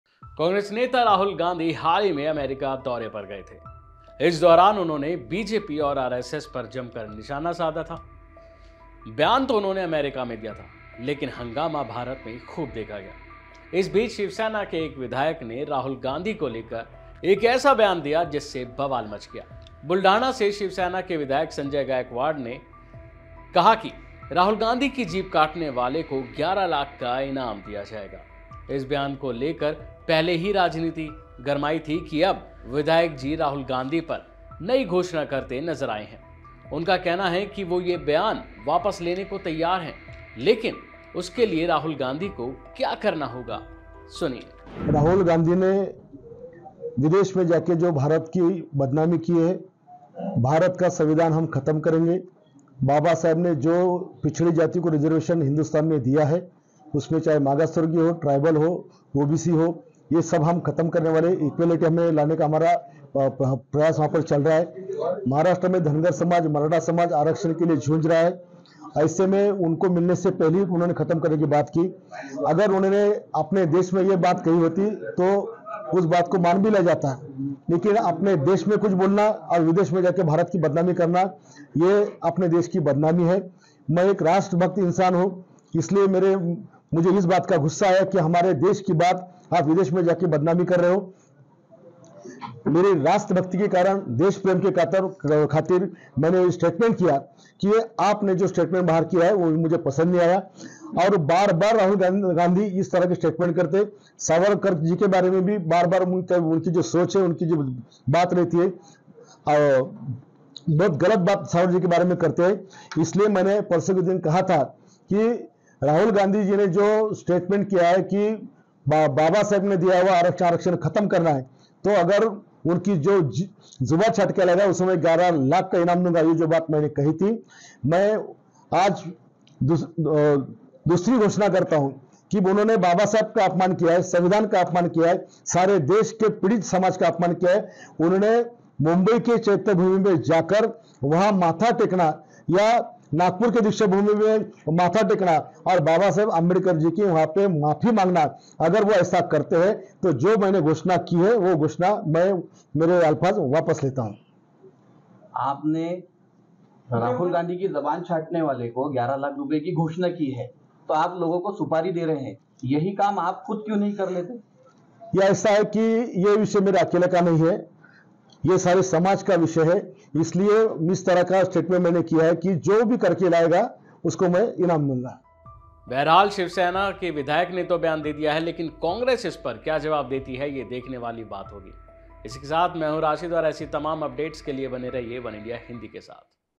Shivsena विधायक Sanjay Gaikwad ने Rahul Gandhi पर अब क्या कह दिया | Maharashtra | वनइंडिया हिंदी
Shivsena Leader on Rahul Gandhi: बुलढाणा से शिवसेना के विधायक (shivsena MLA) संजय गायकवाड (Sanjay Gaikwad) ने कहा कि राहुल गांधी की जीभ काटने वाले को 11 लाख का इनाम (reward of 11 lakhs) दिया जाएगा। इस बयान को लेकर पहले ही राजनीति गरमाई थी कि अब  विधायक जी राहुल गांधी (Sanjay Gaikwad on Rahul gandhi) पर पर नई घोषणा करते नजर आए हैं। उनका कहना है कि वो बयान वापस लेने के लिए तैयार है लेकिन उसके लिए राहुल गांधी को क्या करना होगा सुनिए